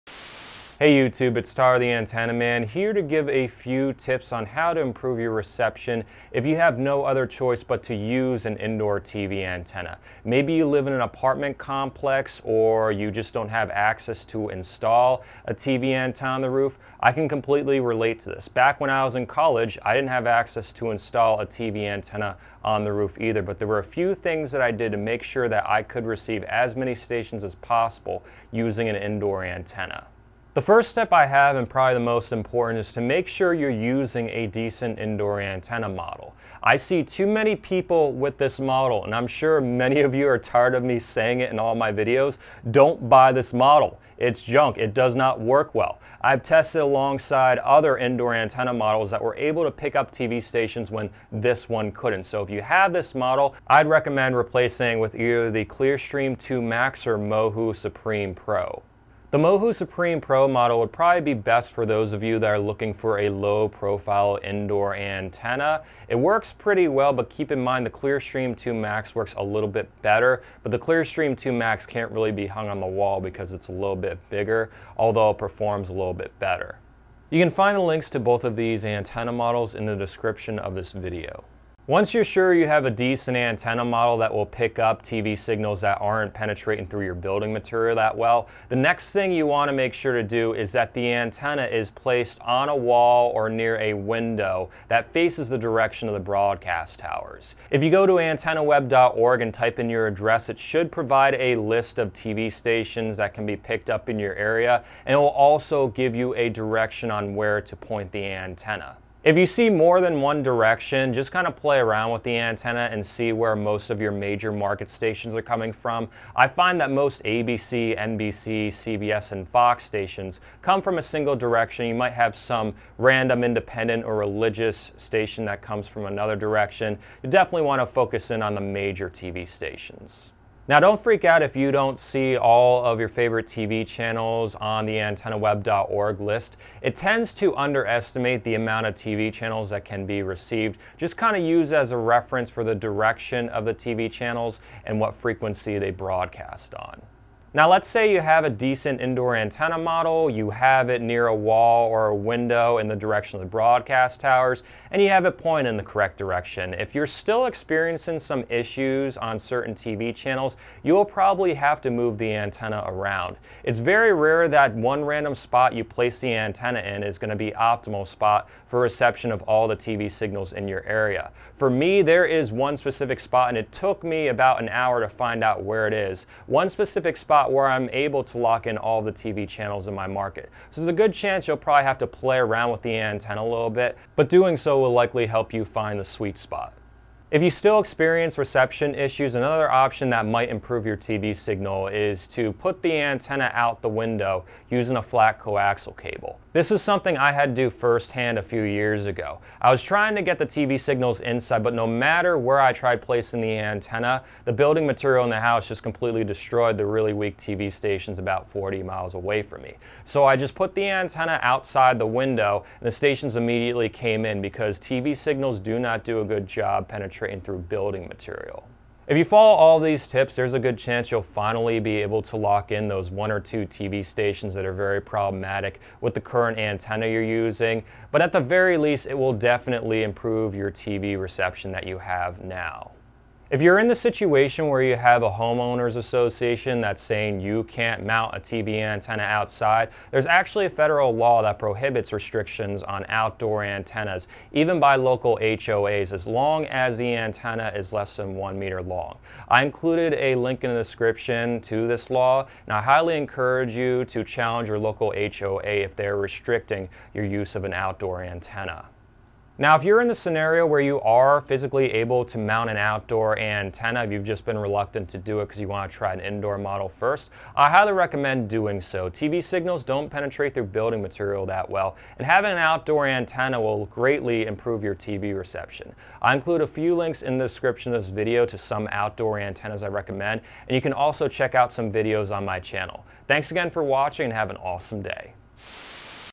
Ways To Improve Indoor TV Antenna Reception
This video explains ways you can improve your reception if you're stuck using an indoor TV antenna. Optimal TV reception is achieved outside but many of you have no choice but to use an indoor TV antenna due to living in an apartment complex or lack of resources to setup an outdoor antenna.

Link to Mohu Supreme Pro:

Link to Flat Cable: